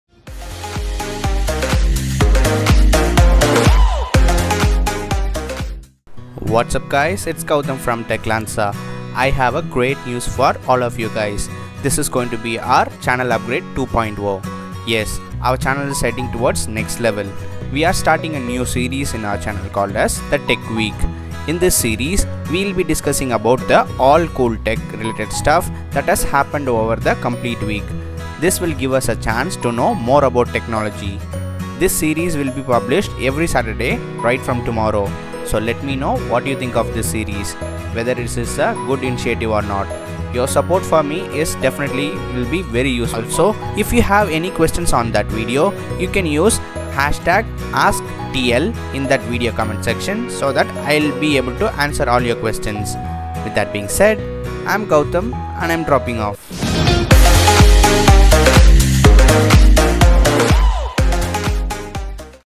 Channel Upgrade 2.0 | New Series "TECH WEEK" | Every Saturday
This is an upgrade video of our Channel. Tech Lancer is starting our new series called " TECH WEEK" which will combine all the technology  video of the complete week.